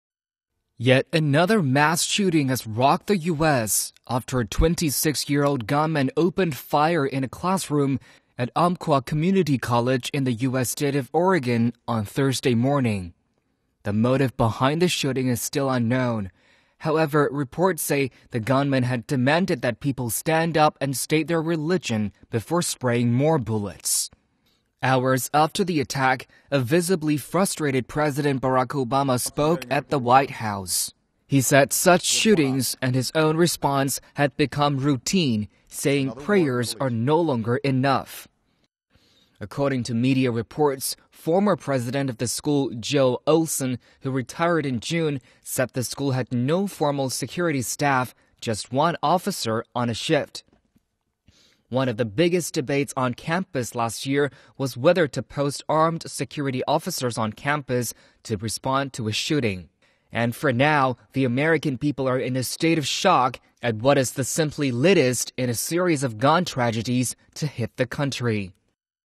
10 killed in US college shooting as Obama calls for tighter gun laws
Ten people have been killed and seven injured in a shooting at a college in the U.S. state of Oregon.

Shortly after the incident, a visibly frustrated President Barack Obama said that "prayers are no longer enough", as he made another call for tighter gun laws.